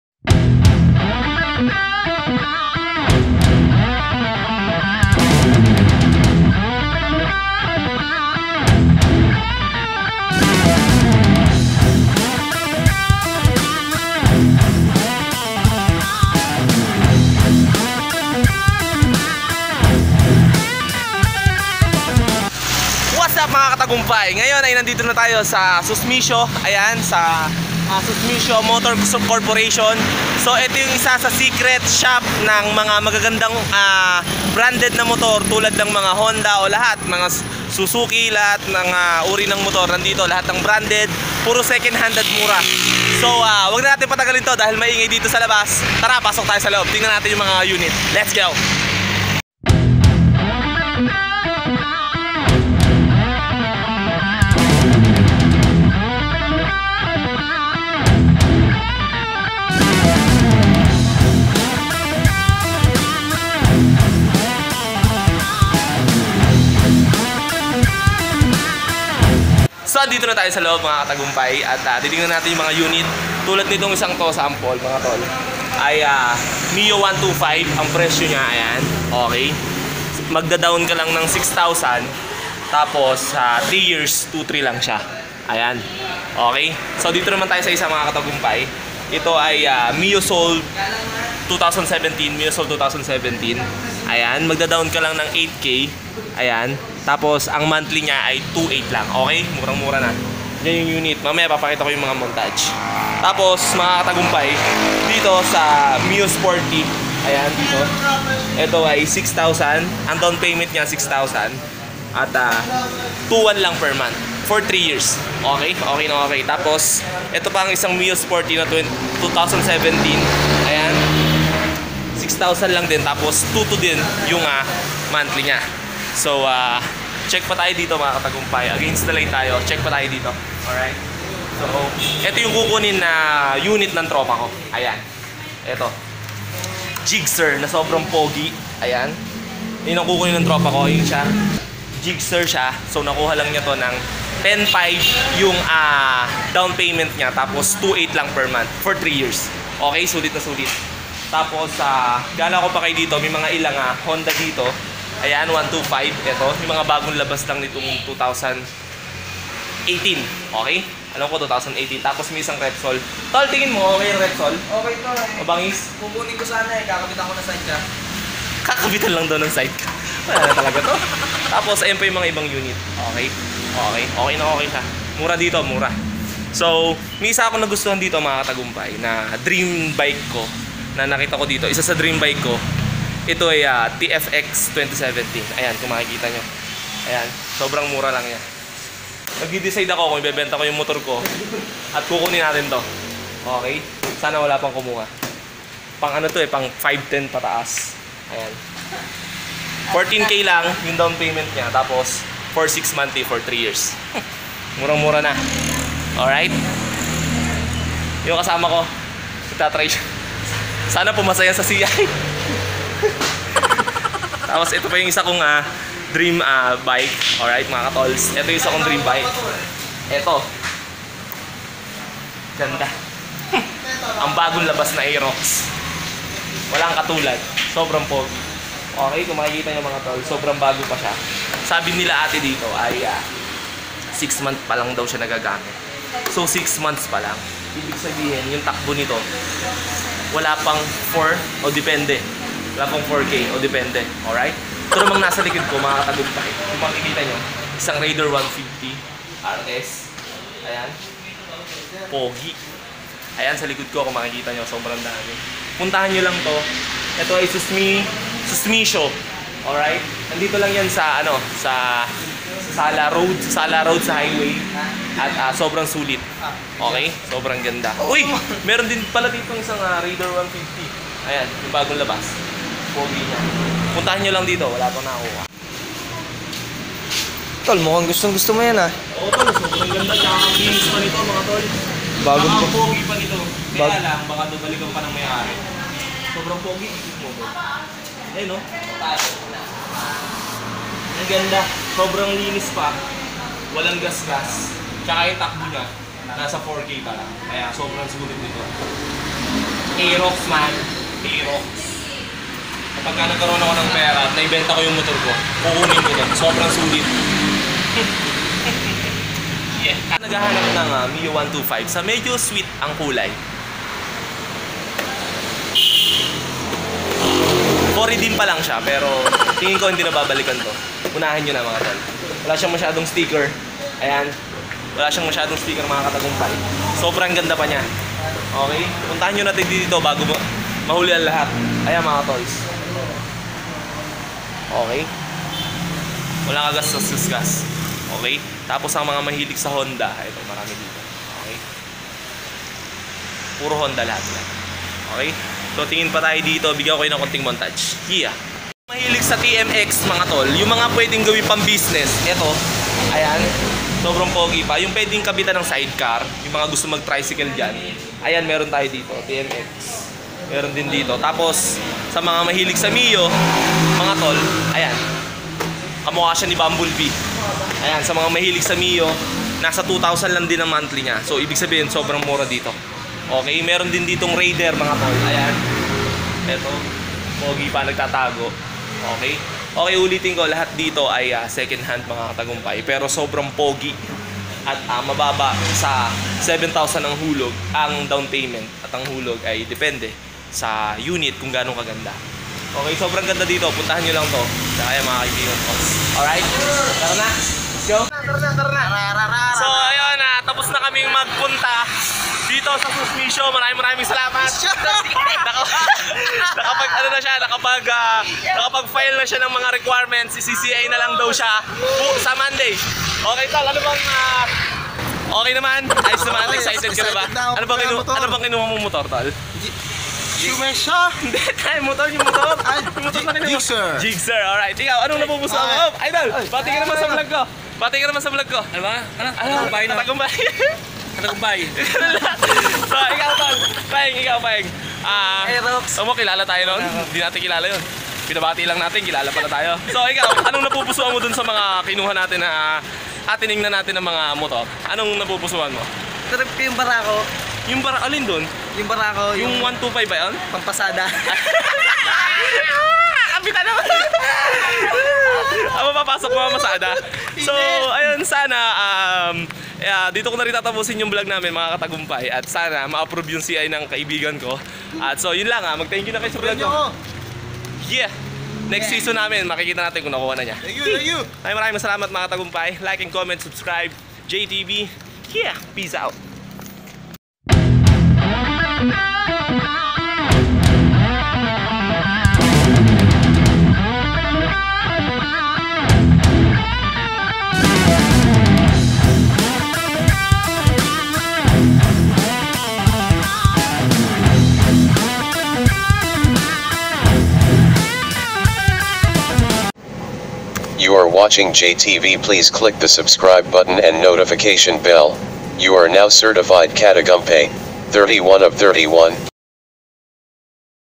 MURANG MGA MOTOR - ( Sikretong Bilihan ) - AEROX 2ND HAND
Sobrang mura ng motor dito mga katagumpay napakadaming mga bagong dating mga tol. kaya puntahan nyo na.. Yung aerox sOBRANG MURA MGA TOLS..

paano pumunta:

Mula sa CABUYAO BAYAN - SAKAY KALANG NG TRICYLCLE tol sabihin mo papunta sa SUSMISHO MOTOR SHOP brgy. sala cabuyao laguna.. Ayos na ayos na yon! oryt!

May gusto kabang ipagawa saakin KATAGUMPAY? Pwede mo akong i-PM OR i -message dito :) FACEBOOK